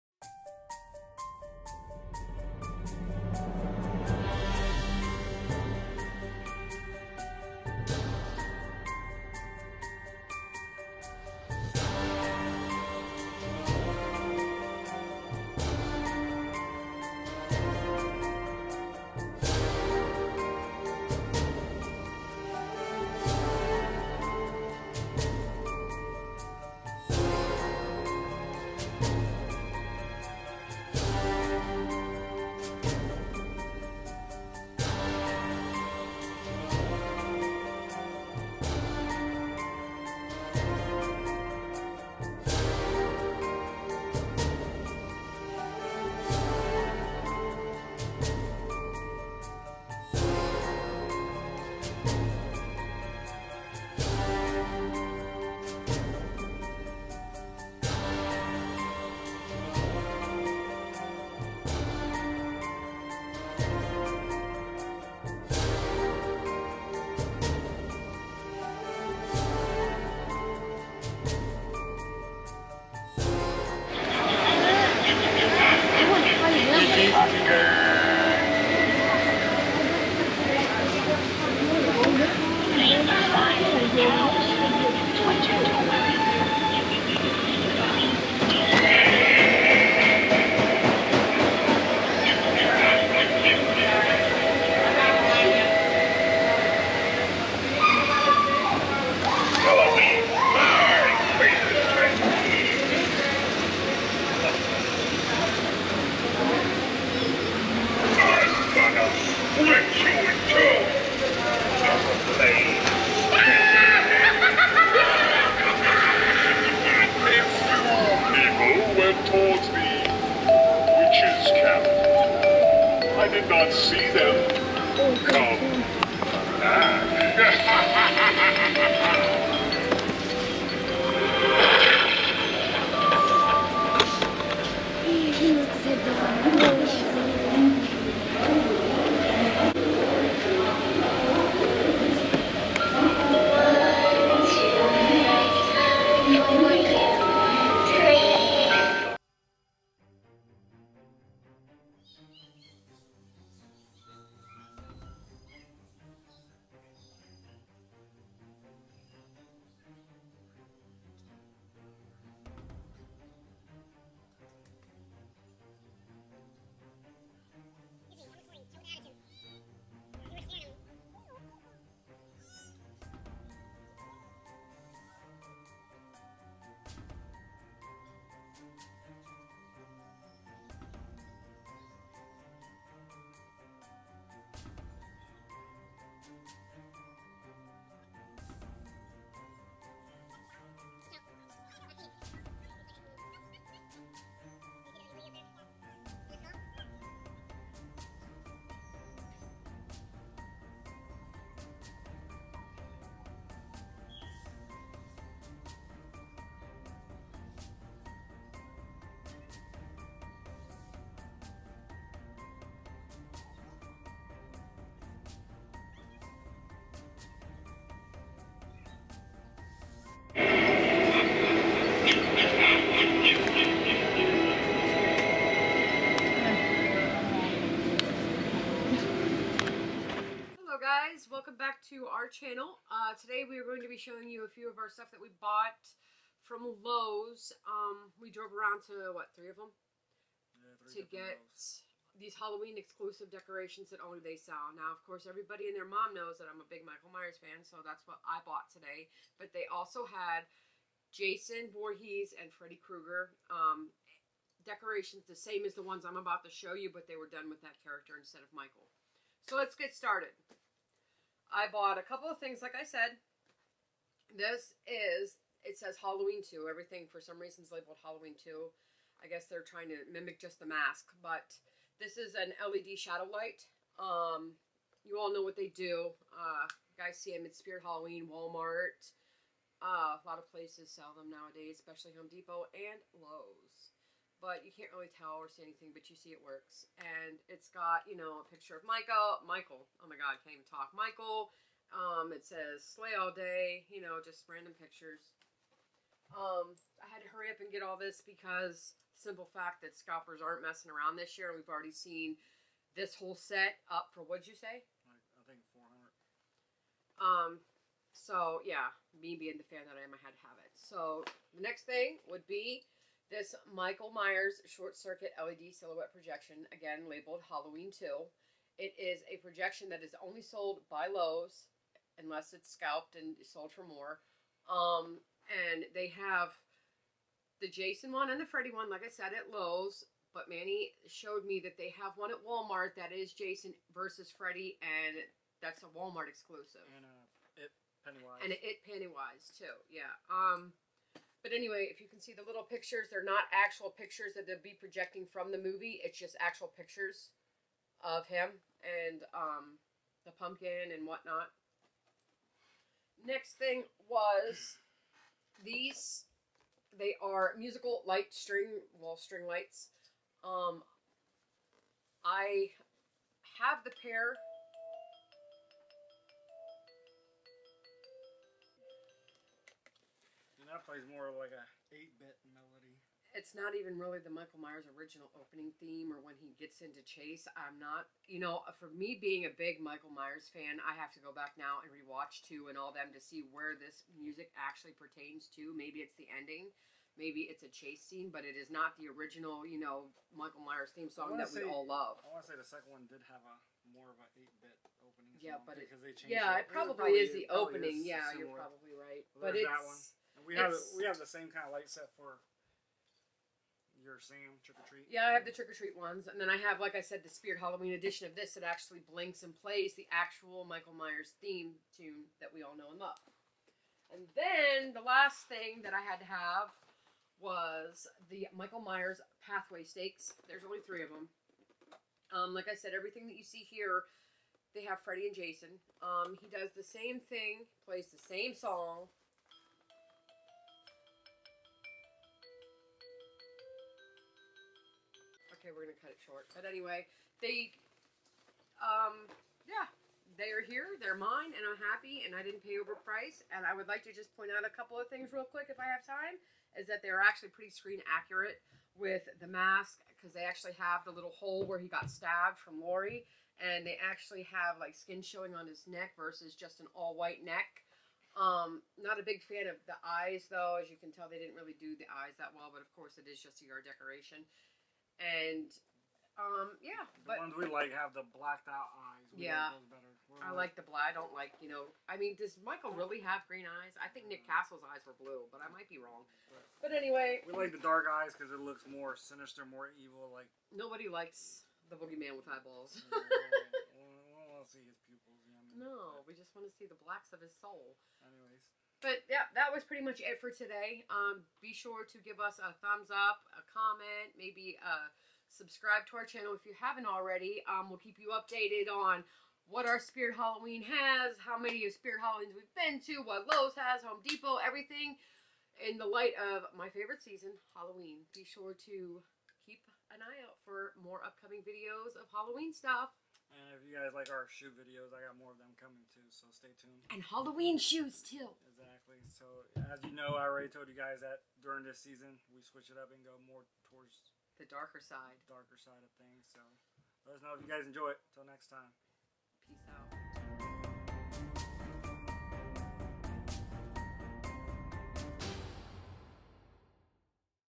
Lowe's Halloween Walkthrough! 2022
Today we are visiting the Lowe's Home Improvement to see what they have in stock for there Halloween decor. We will also show you what we ended up purchasing at this location. This one is at the York, Pa. Hope you guys enjoy.